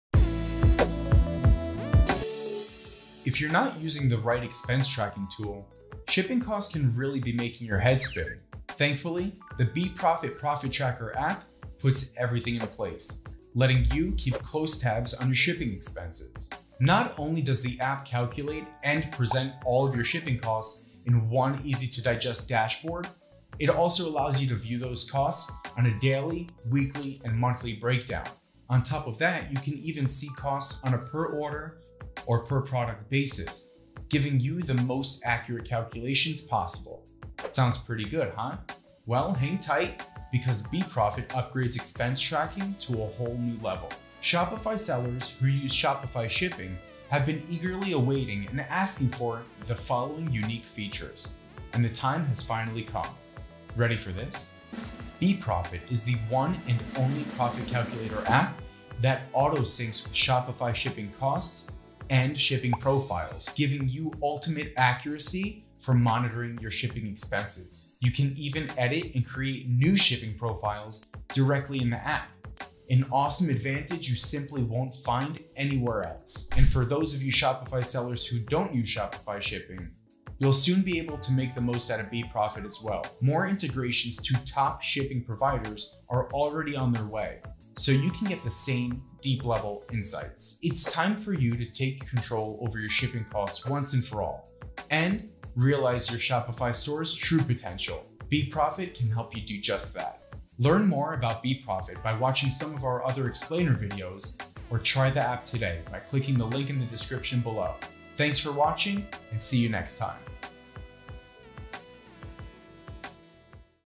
How to Accurately Monitor Shipping Costs (** Shopify Shipping!) [BeProfit - Lifetime Profit Tracker]
Make sense of your Shopify and Wix store's shipping costs with BeProfit - Profit Tracker

If you don’t have the right tool to track expenses, your Shopify and Wix store’s shipping costs can really make your head spin.

Thankfully, the BeProfit - Profit Tracker app puts everything in its place to help you keep a close tab on your shipping expenses.

Not only does the app work as a profit calculator, it also presents your store’s lifetime shipping costs in an easy-to-digest dashboard and allows you to view those shipping costs in a daily, weekly, or monthly breakdown.

You can even filter on a per-product or per-order basis so you can get even more precise calculations of your shipping expenses.

Sounds pretty good, huh? Well, hang tight, because BeProfit lets you track expenses at a whole new level… Shopify sellers have been eagerly asking for this unique feature - and the moment has arrived!

BeProfit is the one-and-only profit calculator app that auto-syncs your Shopify Shipping costs and profiles to give you ultimate accuracy for monitoring your shipping expenses. You can even edit and create new shipping profiles directly in the app - an advantage you simply won’t get anywhere else!

Let’s not forget to mention that the app gives you the ability to export reports for all of your profit and expenses - not just your shipping costs - so you can keep your accounting records in order.

It’s time for you to take control over your Shopify and Wix store’s shipping costs once and for all, and realize your Shopify and Wix store’s true potential! BeProfit can help you do just that.

Learn more about BeProfit - Profit Tracker by watching some of our other explainer videos!